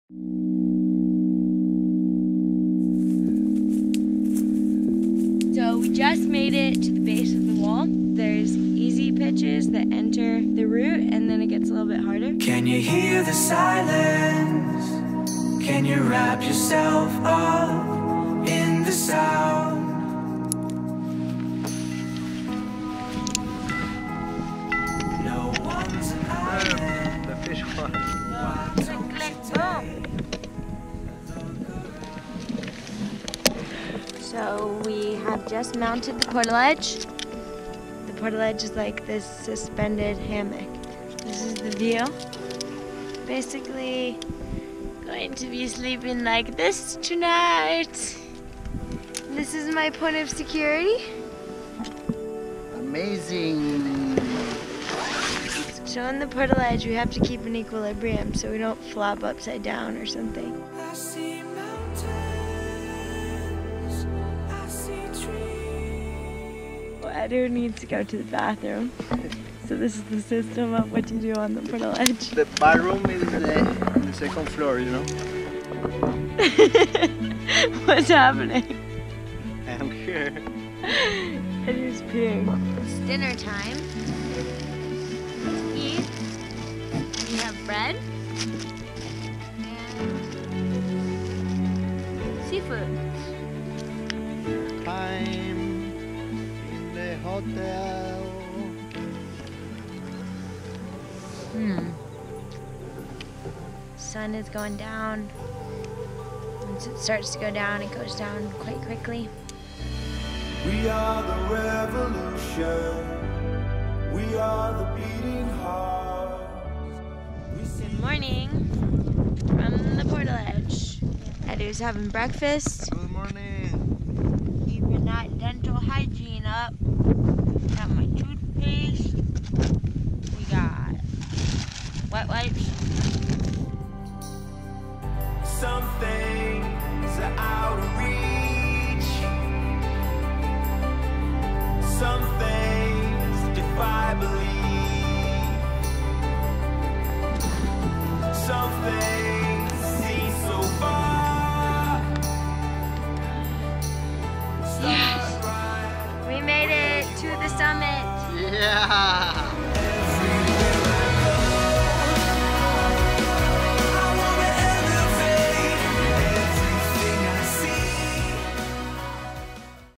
How Climbers Sleep While Climbing Big Walls | with Sasha DiGiulian & Felipe Camargo
►Why bother finding a fancy hotel, when you can do it like pro climber Sasha DiGiulian, choose a thousand stars hotel and spend the night for free?
A first-hand look of what it's like to spend the night at 1,200 feet above the ground on the side of a massive cliff with exclusive footage by pro climber Sasha DiGiulian.